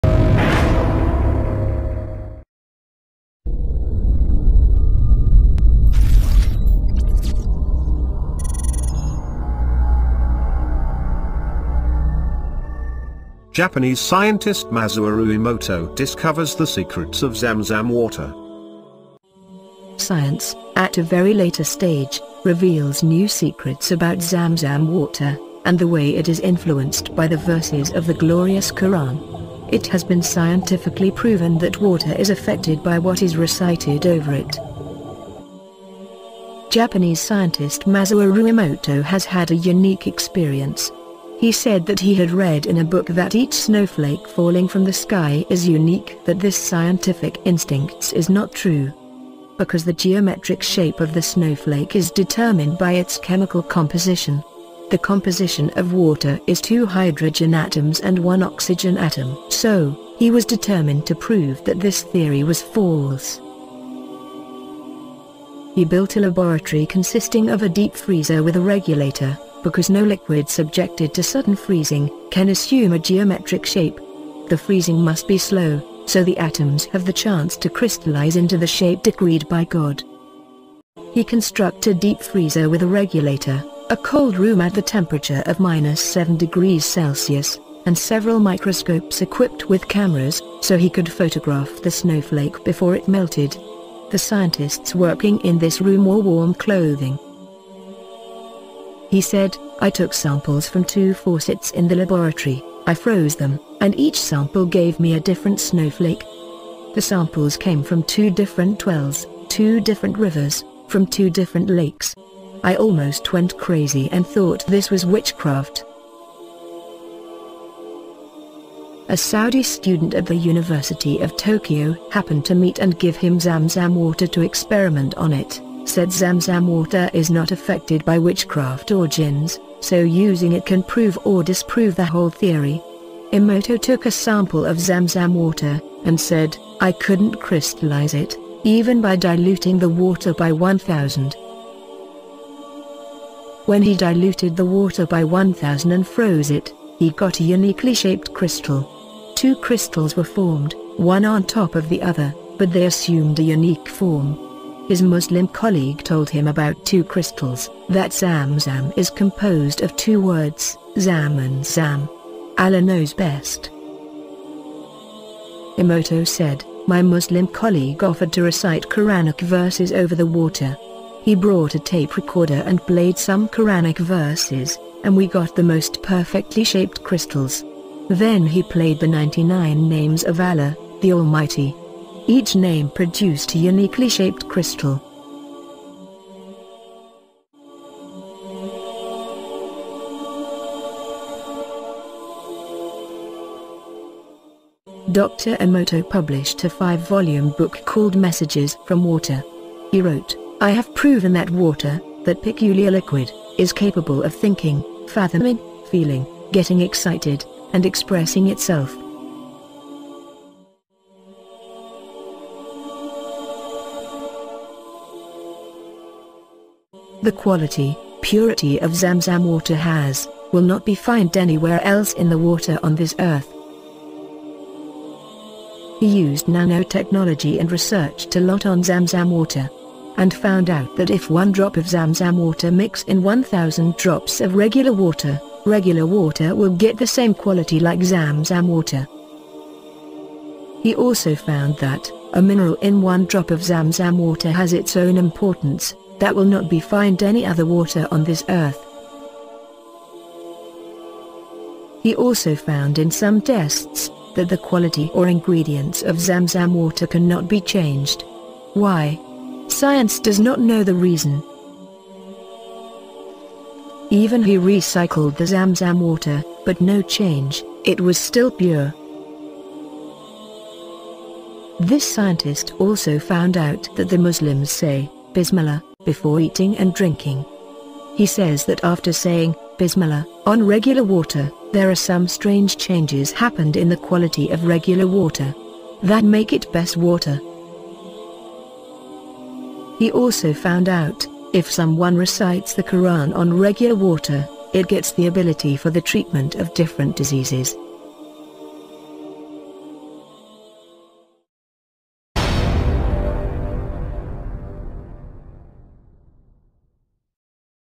Secrets of zamzam water
The Japanese Scientist Masaru Emoto Discovers the secrets of zamzam water